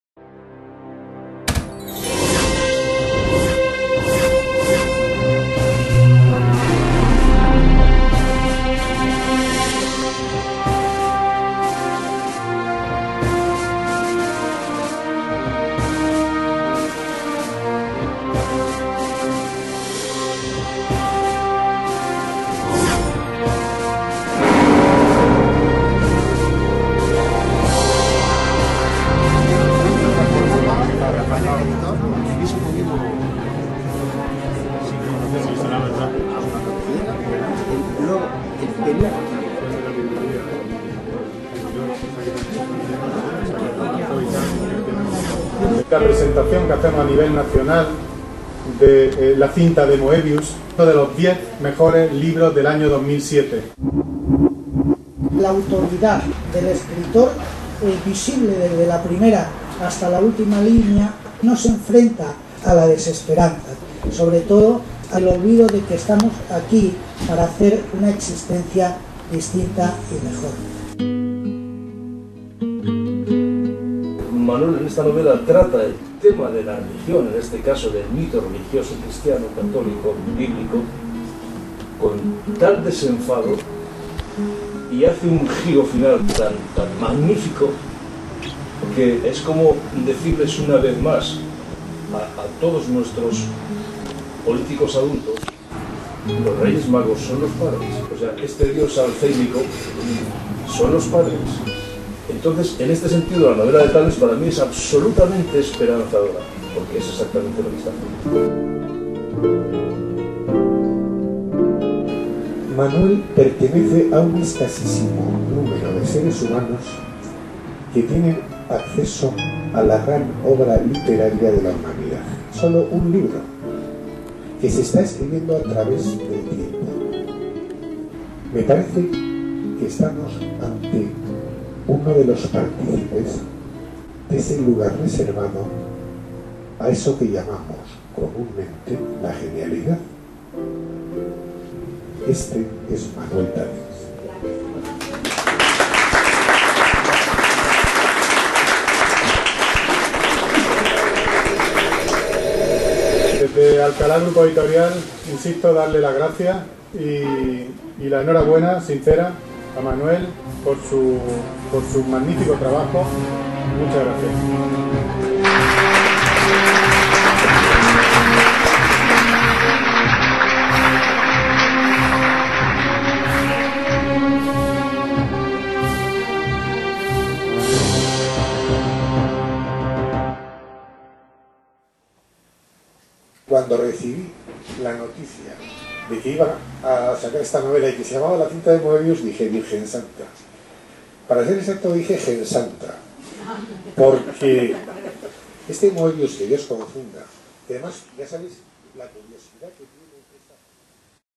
"La cinta de Moebius", de Manuel Talens
Presentación de la novela "La cinta de Moebius", de Manuel Talens, en la librería Fuentetaja de Madrid el 31 de enero de 2008, con Antonio Fraguas "Forges", Abbé Nozal, Manuel Talens, Rafael Ceballos y Alfonso Fernández Burgos.